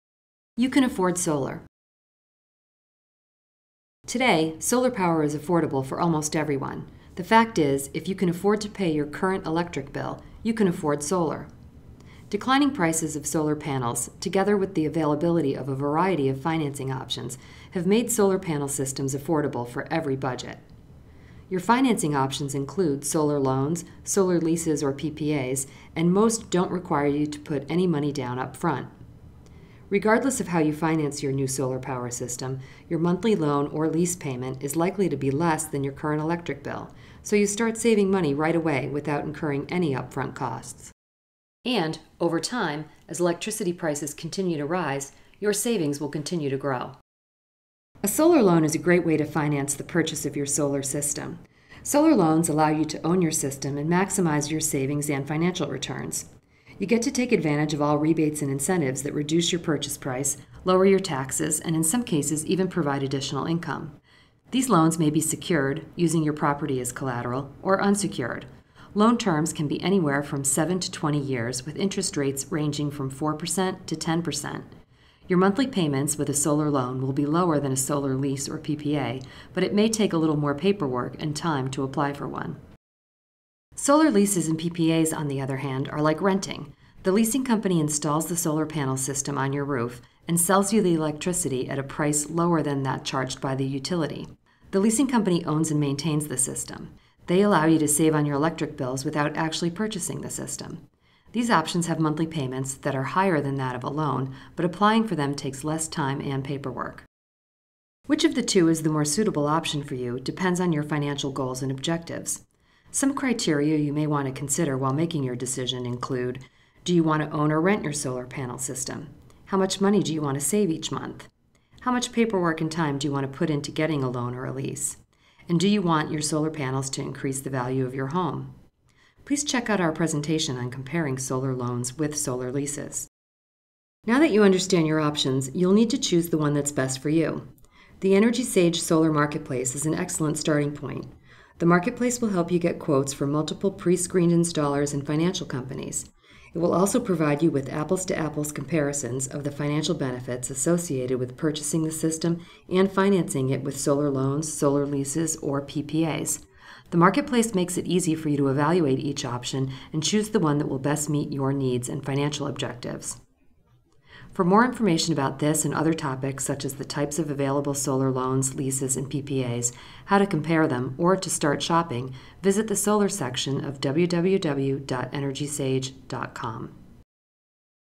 Solar Panels for Home- Cost of Solar Panels
Wondering what solar panels cost? Curious what you should know about home solar panels? Our online marketplace will help you understand all the tips to successful solar financing. A wide variety of financing options—solar loans, solar leases and solar PPAs—make solar power affordable for everyone For more detailed information,